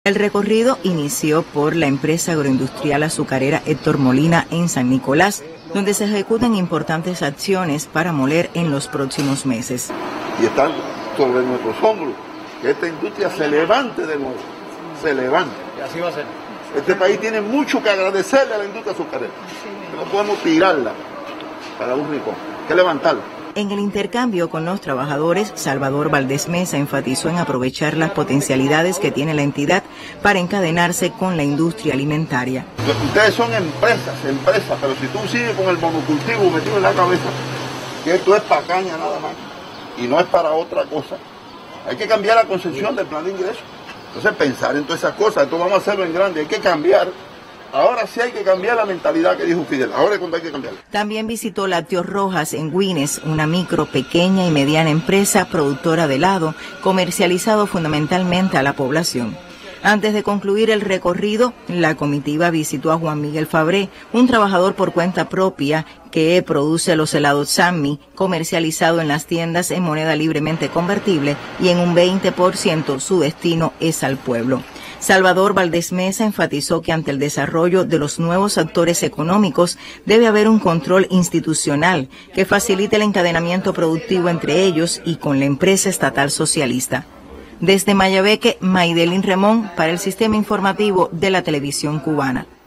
Recorrió vicepresidente de Cuba centros de interés económico de Mayabeque
El miembro del Buró Político y vicepresidente de la República de Cuba, Salvador ValdésMesa, visitó centros de interés económico de Mayabeque. Le acompañaron la miembro del Comité Central del Partido y su primera secretaria en ese territorio, Yuniasky Crespo Baquero.